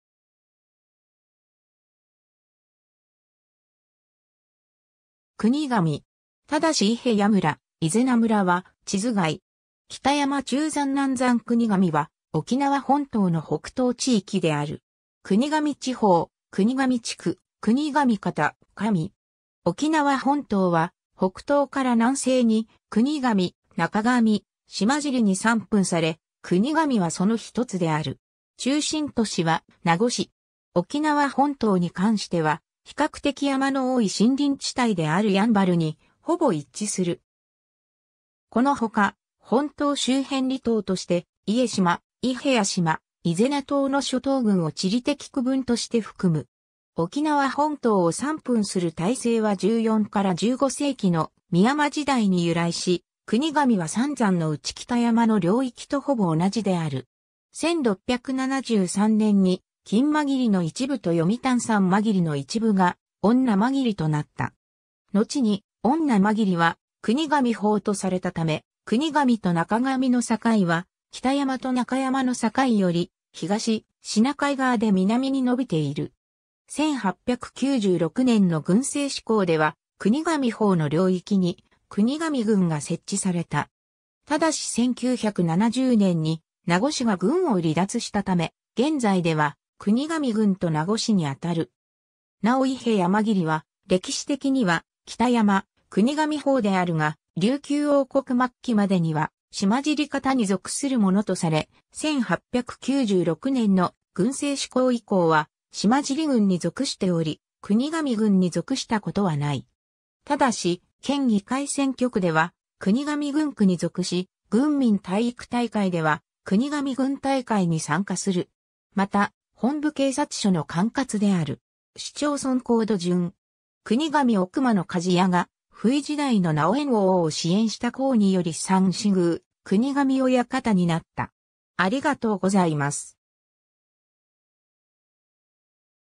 国頭
ただし伊平屋村・伊是名村は地図外。
 北山（山北） 中山（山中） 南山（山南） 国頭（くにがみ、くんじゃん）は、沖縄本島（および周辺島嶼）の北東地域である。
国頭地方、国頭地区、国頭方、カミ。
 沖縄本島は北東から南西に国頭・中頭・島尻に三分され、国頭はその1つである。
中心都市は名護市。
 沖縄本島に関しては、比較的山の多い森林地帯である山原（やんばる）にほぼ一致する。
この他、本島周辺離島として、伊江島、伊平屋島、伊是名島の諸島群を地理的区分として含む（行政区分としては後述の通り、伊江村だけが国頭に含まれる）。
 沖縄本島を三分する体制は14～15世紀の三山時代に由来し、国頭は三山のうち北山の領域とほぼ同じである。
 1673年に金武間切の一部と読谷山間切の一部が恩納間切（のちの恩納村）となった。
のちに恩納間切は国頭方とされたため、国頭と中頭の境は北山と中山の境より東シナ海側で南に伸びている。
 1896年の郡制施行では国頭方の領域に国頭郡が設置された。
ただし1970年に名護市が郡を離脱したため、現在では国頭郡と名護市にあたる。
 なお伊平屋間切（伊是名村と伊平屋村）は歴史的には北山・国頭方であるが、琉球王国末期までには島尻方に属するものとされ、1896年の郡制施行以降は島尻郡に属しており、国頭郡に属した事はない。
ただし県議会選挙区では国頭郡区に属し、郡民体育大会では国頭郡大会に参加する。
また本部警察署の管轄である。
 市町村コード順。
 国頭奥間（現在の国頭郡国頭村字奥間）の鍛冶屋が布衣時代の尚円王を支援した功により三司宮国頭親方になった。